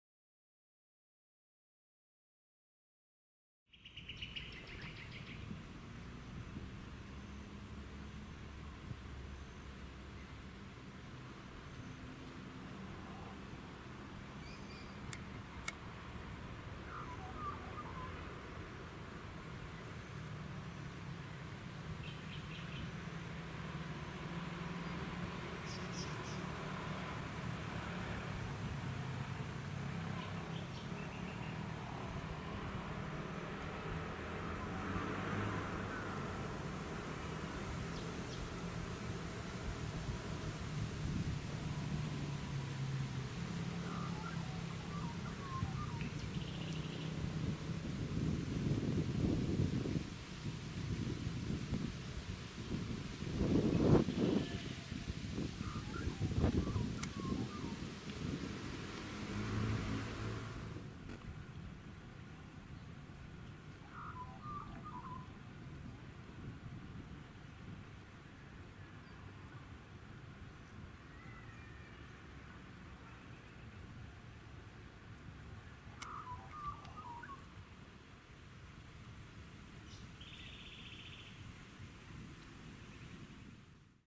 Cotton Pygmy-goose
Cotton Pygmy-goose. Scientific name: Nettapus coromandelianus. Filmed in the Sunshine Coast hinterland, Queensland, in August 2019. This video includes the sound of birdsong, motor vehicles and the wind